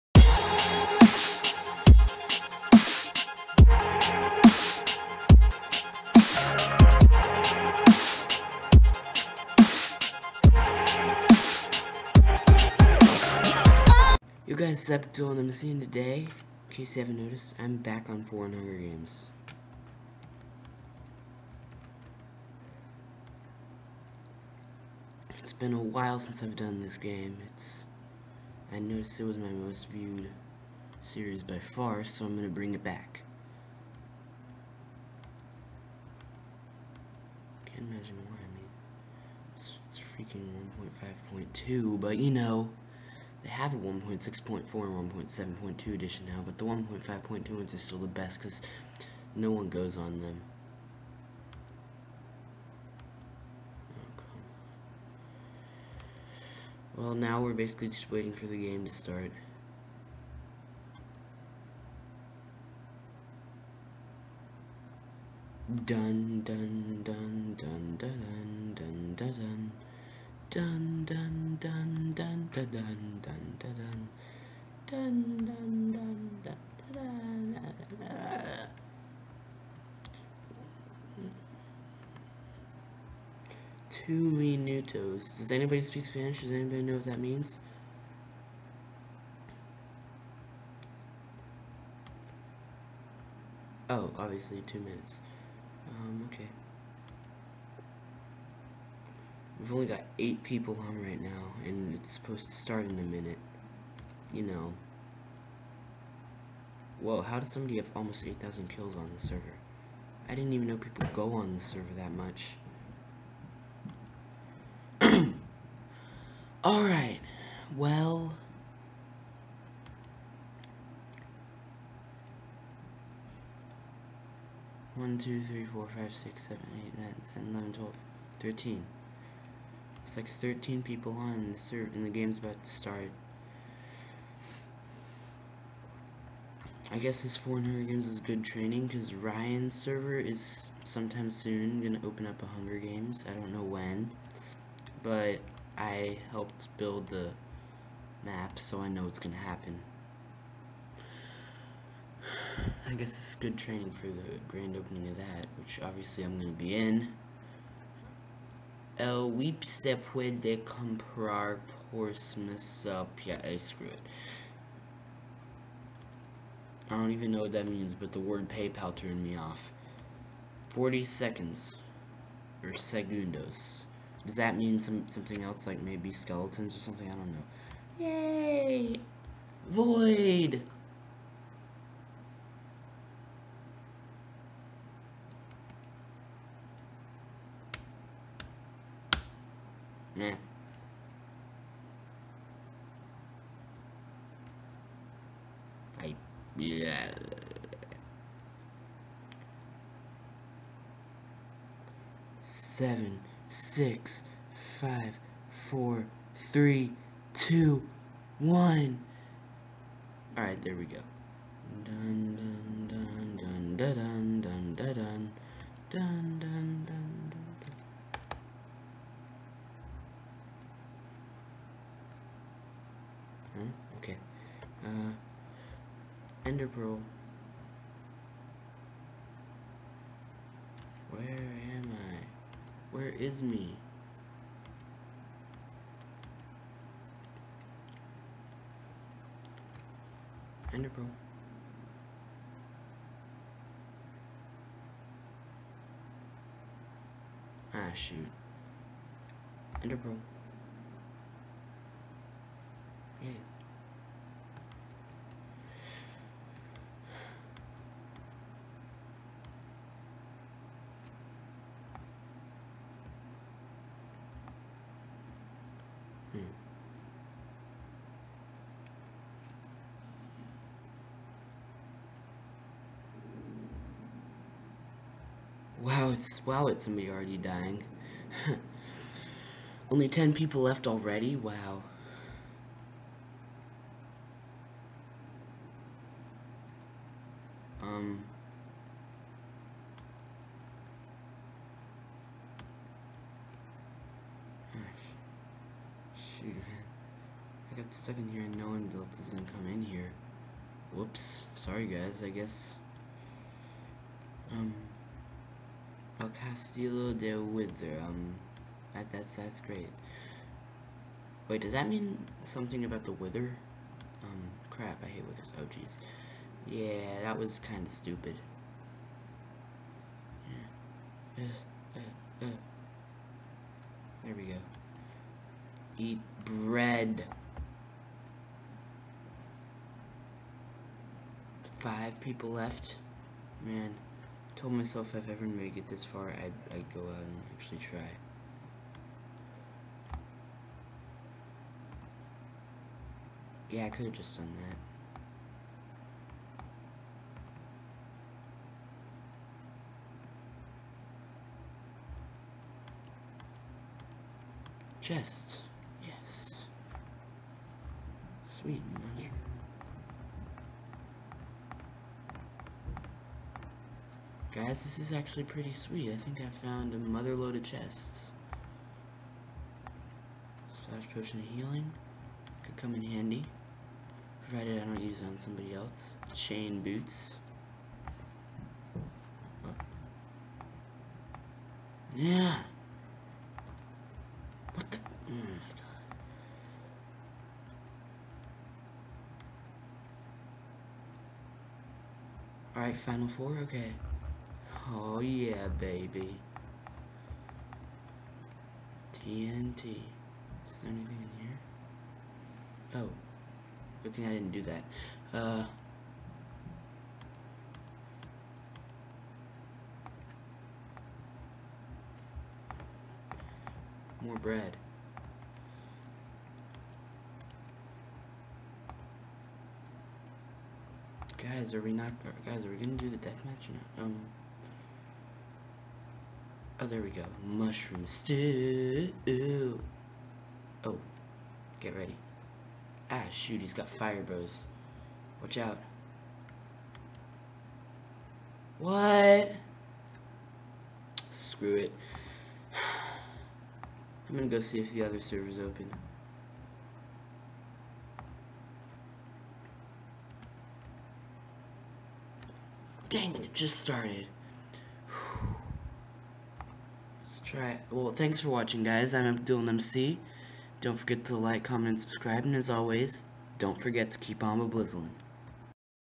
Foreign Hunger Games Episode 5: BRINGING IT BACK!!!!!!!
After all this time, I decided to bring my most popular series back.  I make it to the final four today, but then something interesting happens...